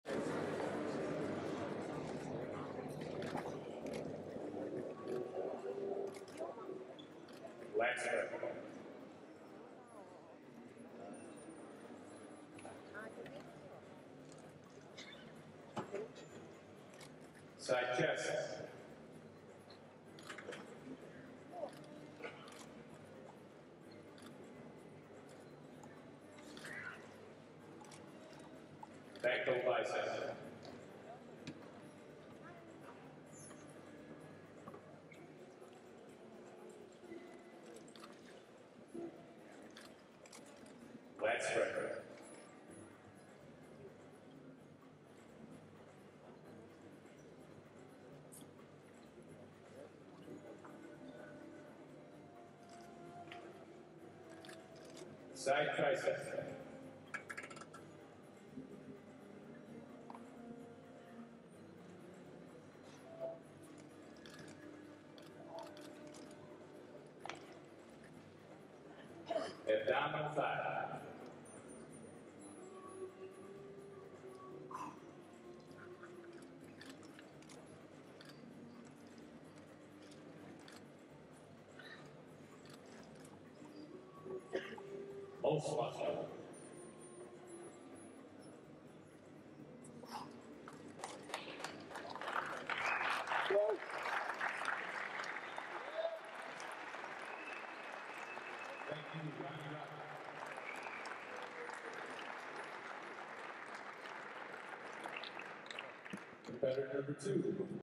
Ronny Rockel 2011 Arnold Prejudging Raw Footage
Excuse the footage. We were not at this contest to shoot Rockel specifically and you can see that we were just setting up and getting our camera adjusted when the individual posing began.

Still, a fan of the channel, and a big fan of Rockel, requested this clip and I was happy to oblige.